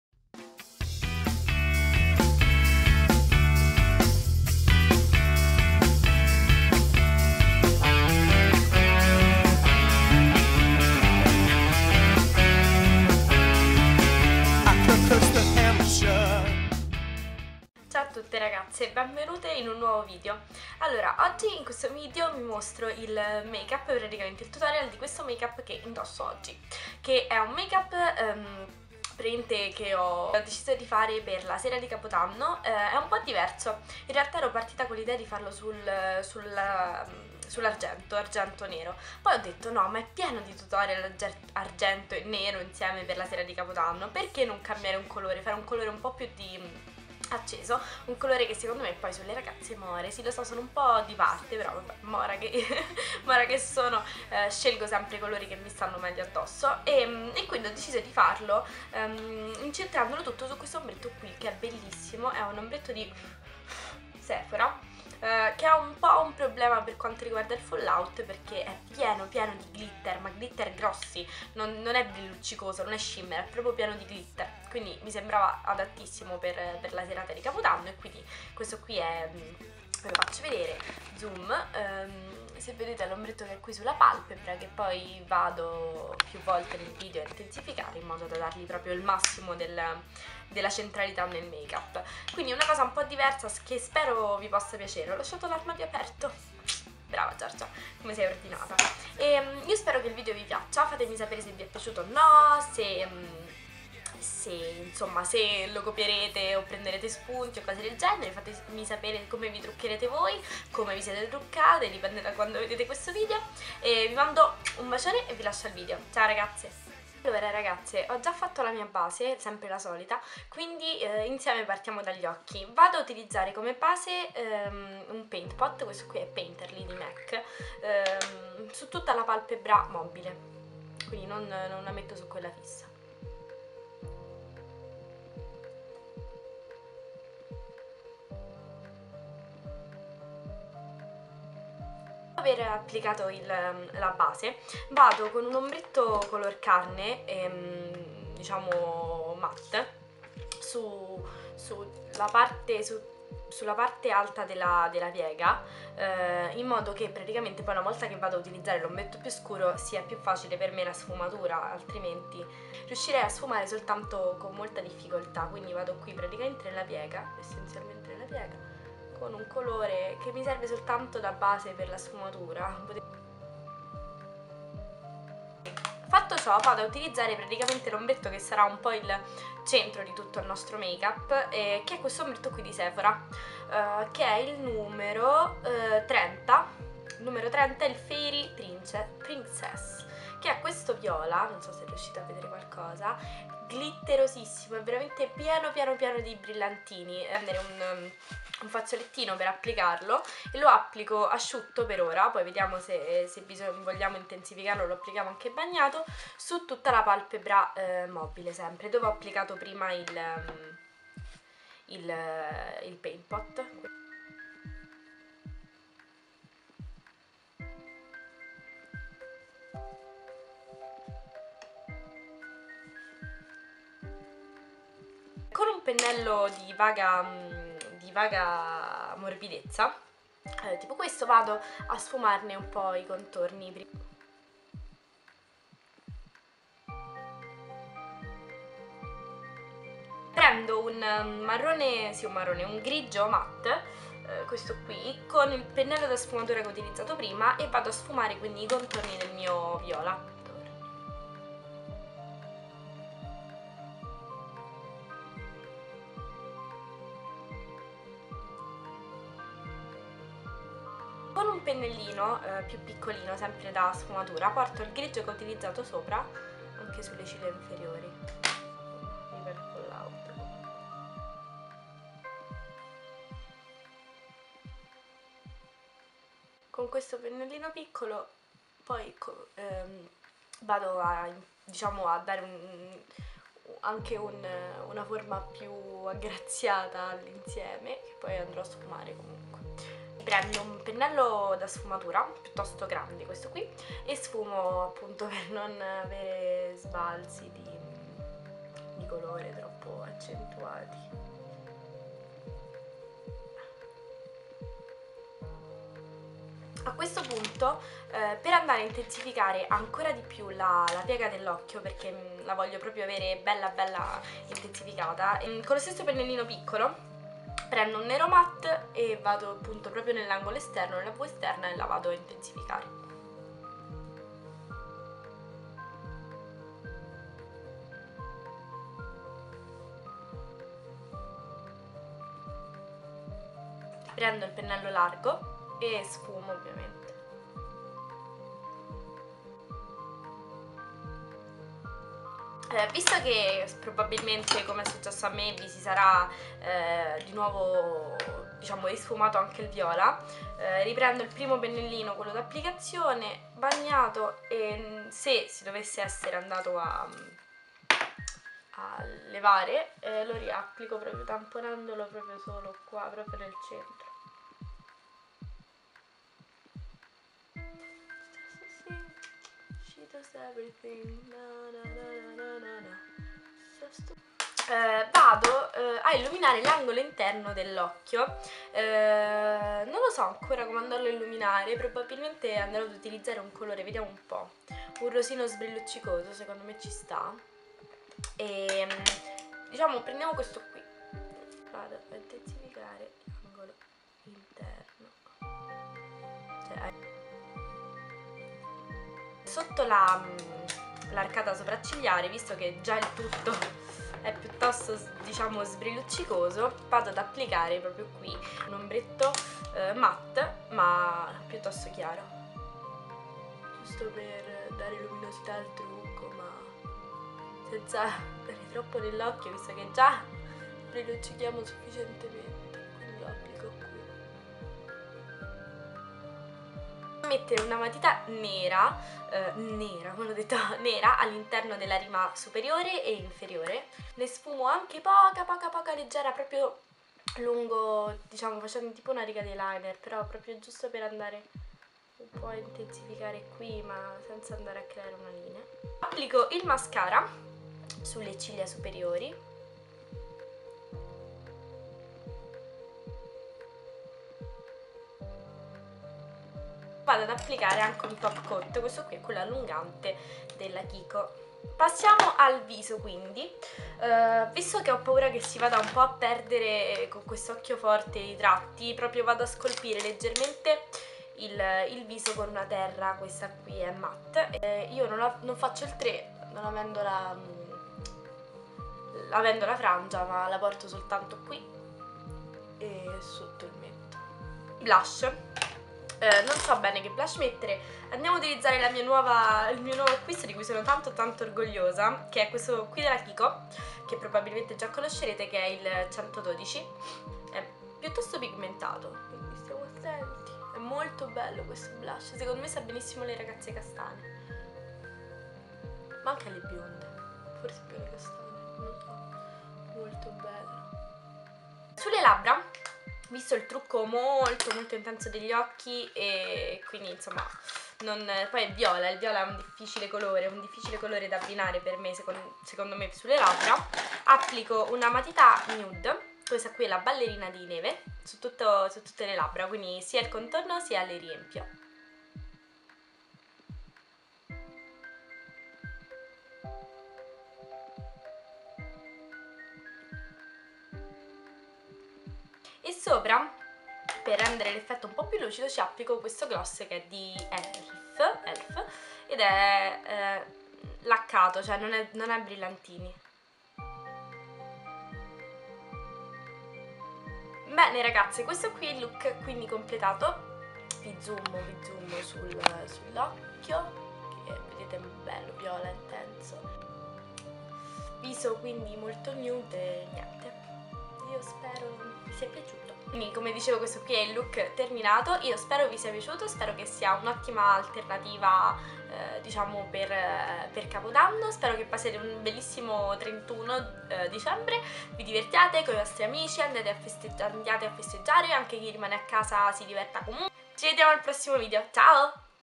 Purple New Year's Eve - Makeup Tutorial
Ciao ragazze ecco la mia proposta makeup per la sera di capodanno! Vi piace? Fatemelo sapere con un commento! un bacione!